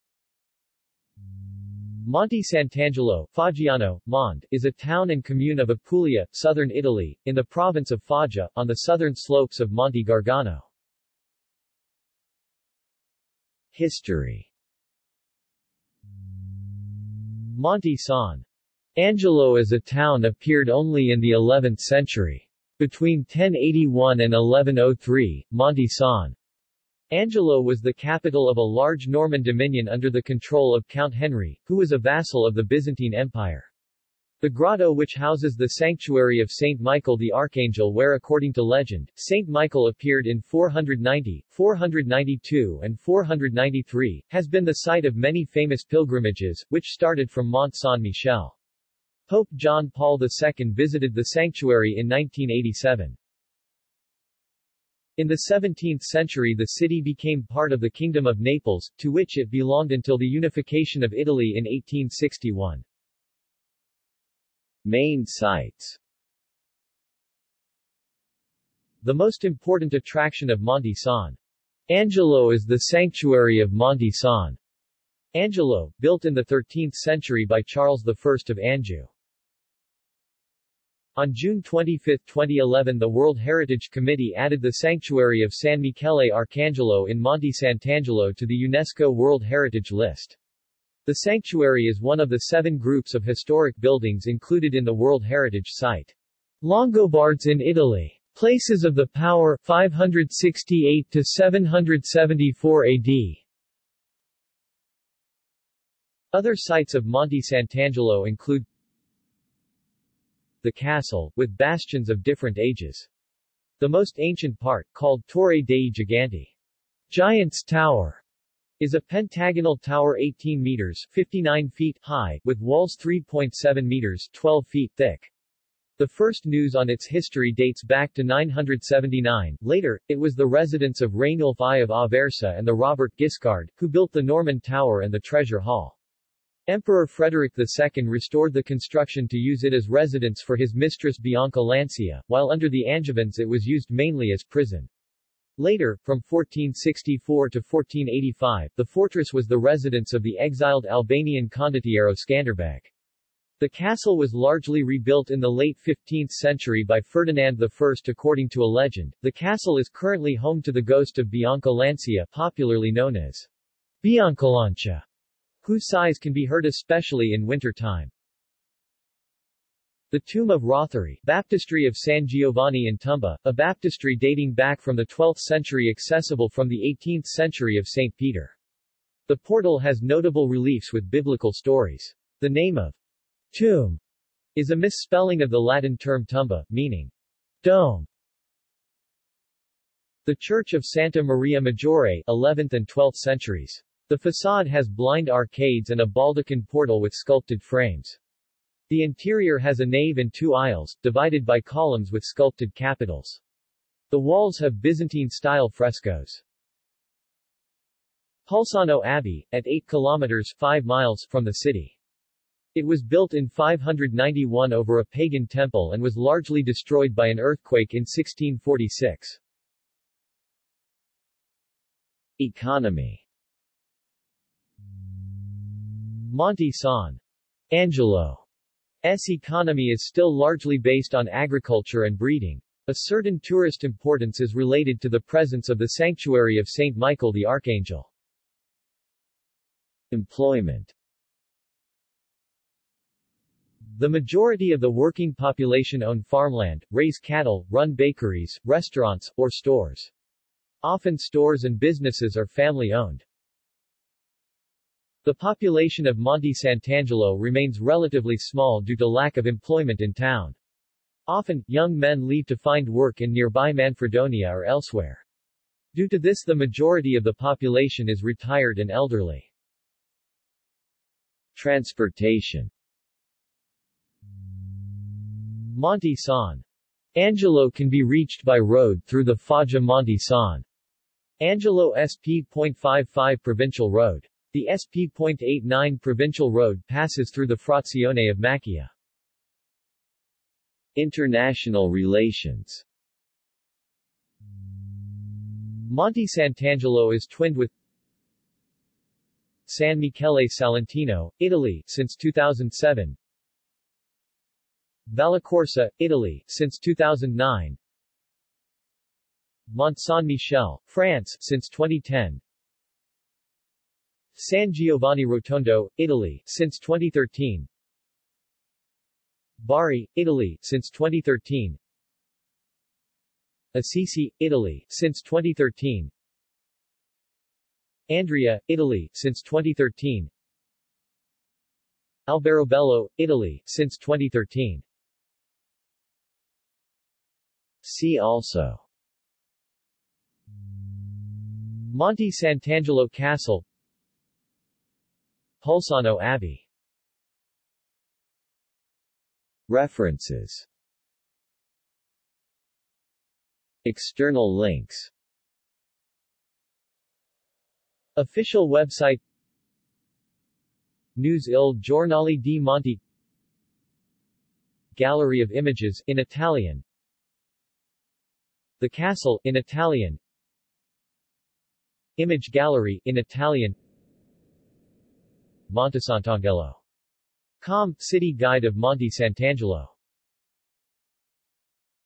Monte Sant'Angelo | Wikipedia audio article
This is an audio version of the Wikipedia Article:
Monte Sant'Angelo

SUMMARY
Monte Sant'Angelo (Foggiano: Mónde) is a town and comune of Apulia, southern Italy, in the province of Foggia, on the southern slopes of Monte Gargano.